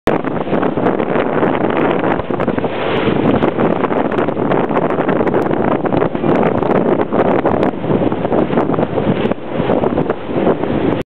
Little Boat Bobbing Along
A boat next to our whale watching boat.